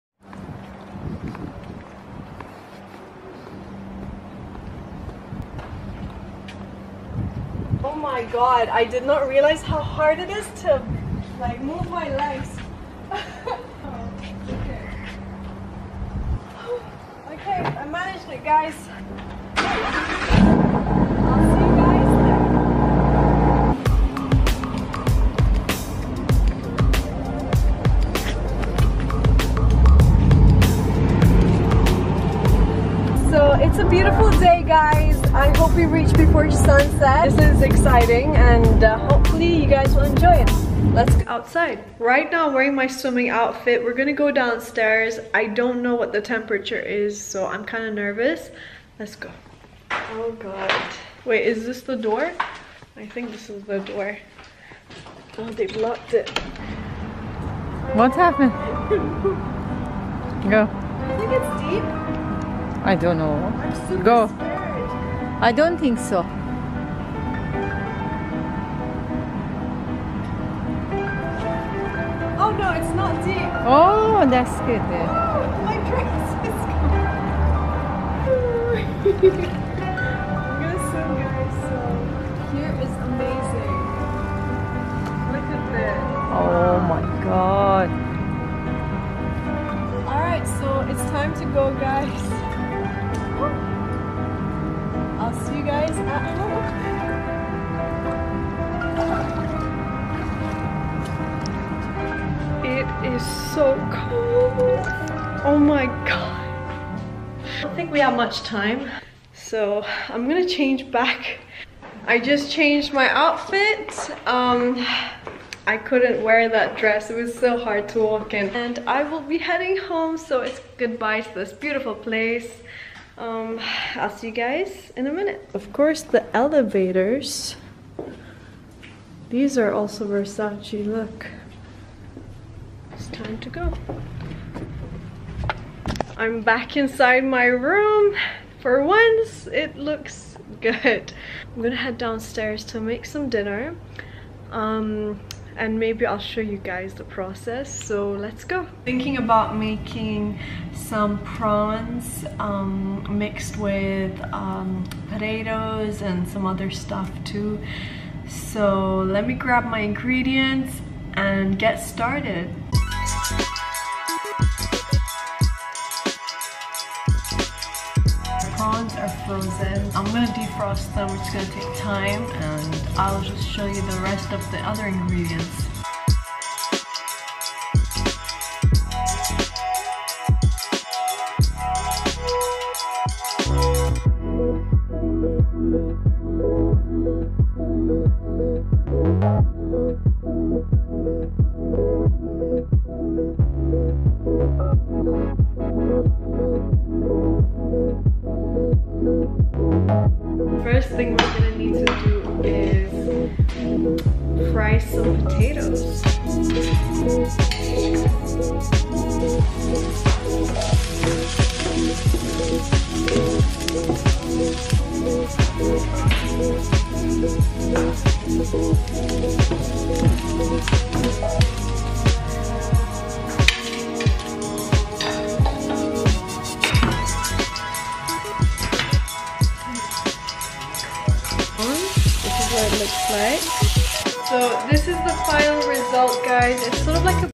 Beautiful Lana Rose driving Lamborghini and Swimming with Sexyoutfit
Beautiful Lana Rose driving Lamborghini and Swimming with Sexy outfit

Thanka for the Watching this Video...